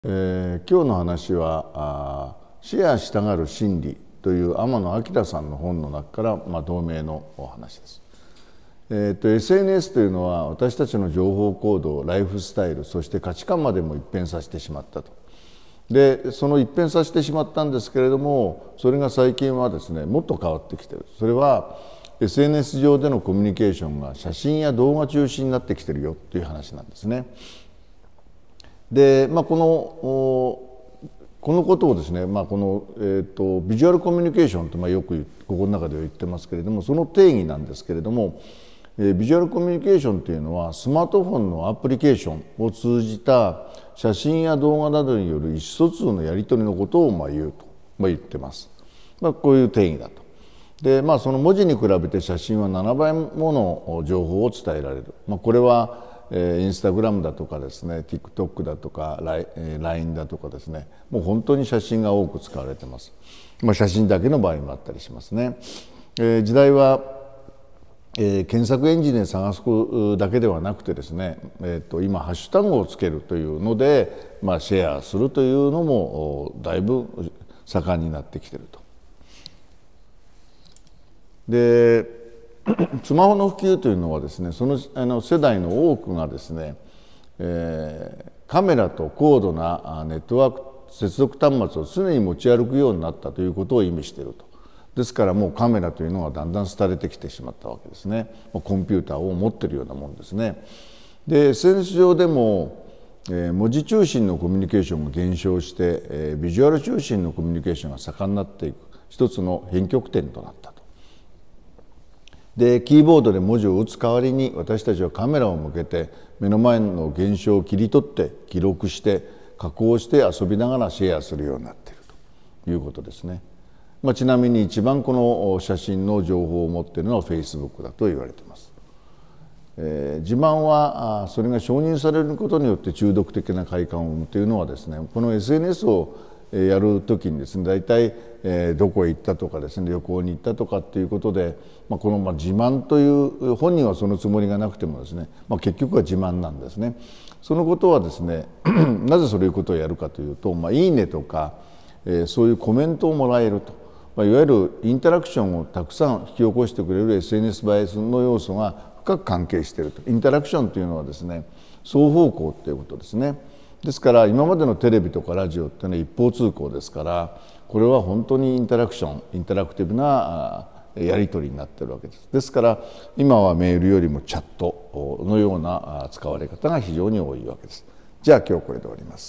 【シェアしたがる心理】（天野彬・あきら）
1. ＳＮＳは私たちの情報行動、ライフスタイル、そして価値観までも一変させてしまった＊そのＳＮＳ上でのコミュニケーションが写真や動画中心になってきている
2.ビジュアルコミュニケーション とは、「スマートフォンのアプリケーション」を通じた「写真や動画などによる意思疎通のやりとり」＊文字に比べ、写真は7倍もの情報を伝えられる
 3.（時代は）検索エンジンで探すことでけでなく、ハッシュタグをユーザー同士でつけてシェアしたコンテンツをＳＮＳの中で探すように変化している ＊  ＳＮＳにおけるシェア文化を考えるうえで欠かせない視点

4.ハッシュタグをつけてコンテンツをシェアすること、そして「手繰る」ように情報を集めていくことをいう二つの言葉をかけた造語が「タグる」
◆スマホの普及は、その世代の多くがカメラと高度なネットワーク接続端末をつねに持ち歩くようになったことを意味している ＊ＳＮＳ上でも、文字中心のコミュニケーションが減少し、ビジュアル中心のコミュニケーションが盛んになっていく、一つの変曲点となった ＊キーボードで文字を打つ代わりに、私たちはカメラを向けて目の前の現象を切り取り、記録し、加工して遊びながら、シェアするようになっている
◆ 自慢はそれが承認されることによってこそ中毒的な快感を生む⇒その快感は私たち一人ひとりのアイデンティティを刺激する＊私たちのシェアしたがる心理には、ＳＮＳでのいいね！やコメントなどインタラクションをたくさん引き起こしてくれる「ＳＮＳ映え」の要素が深く関係している＊ インタラクションとは、英語の inter（相互に）とaction（作用）を合成したもので、アクションとリアクションが起こることで、一方通行ではない双方向の状態をいう＊ 今までのテレビやラジオといったメディアは、ほとんどが一方通行だ。しかし、ＳＮＳはインタラクティブで、対話するような感じ＊最近はビジネスでも、メールではなく、チャットの方が多く使われる（画像が多い） ＊ＳＮＳには自慢や承認欲求が横行している⇒それが嫉妬や羨望（せんぼう）を生む